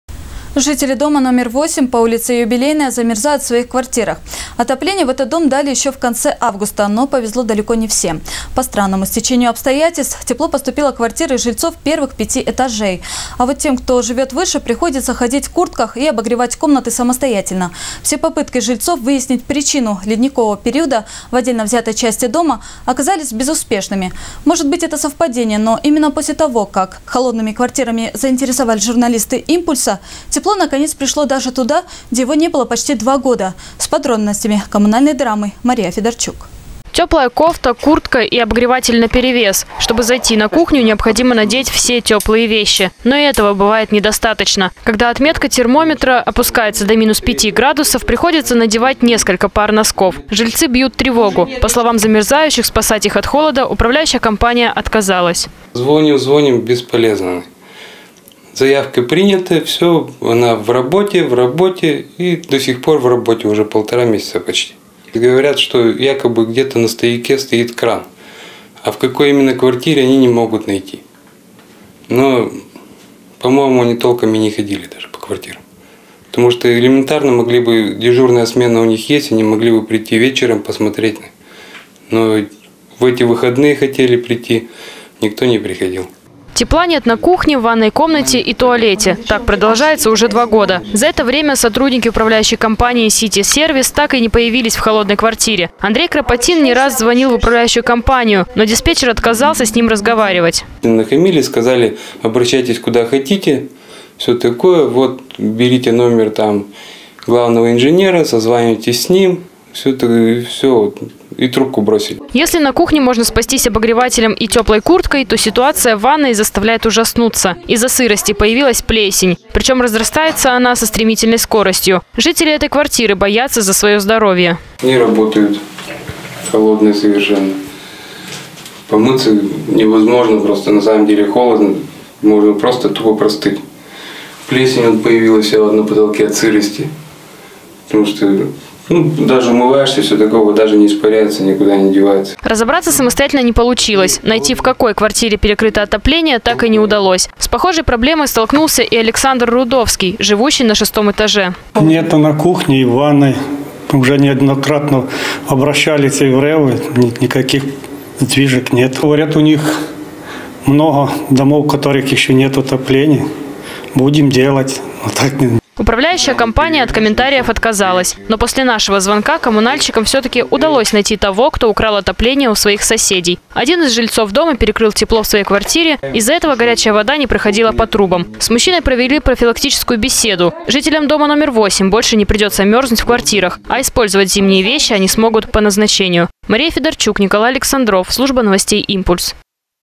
Жители дома 8 по улице Юбилейная замерзают без отопления.
Жители дома №8 по улице Юбилейная замерзают в своих квартирах. Отопление в этот дом дали ещё в конце августа, но повезло далеко не всем. По странному стечению обстоятельств тепло поступило в квартиры жильцов первых пяти этажей. А вот тем, кто живёт выше, приходится ходить в куртках и обогревать комнаты самостоятельно. Все попытки жильцов выяснить причину «ледникового периода» в отдельно взятой части дома оказались безуспешными. Может быть, это совпадение, но именно после того, как холодными квартирами заинтересовались журналисты «Импульса», тепло наконец пришло даже туда, где его не было почти два года. С подробностями коммунальной драмы Мария Федорчук.